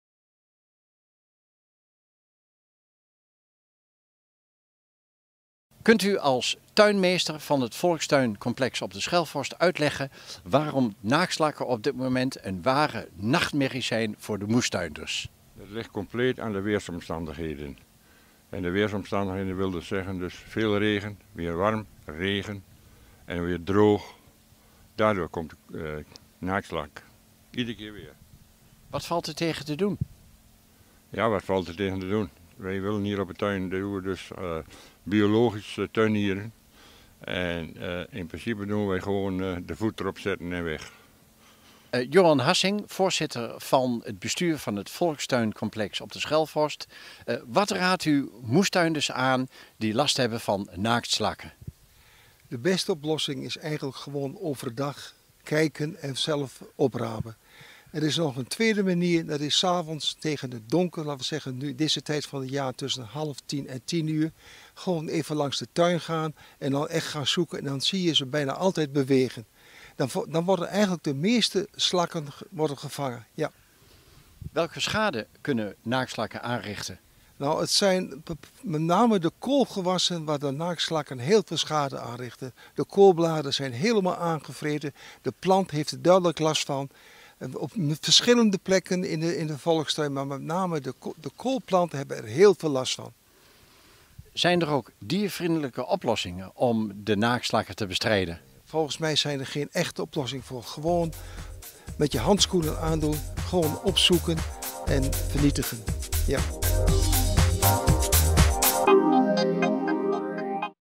Naaktslakken nachtmerrie voor volkstuinders
De vele naaktslakken zijn op dit moment een ware nachtmerrie voor moestuinders op volkstuincomplex Schelfhorst. Maar ook bezitters van siertuinen kunnen over de vraatzucht van deze slijmerige wormpjes meepraten.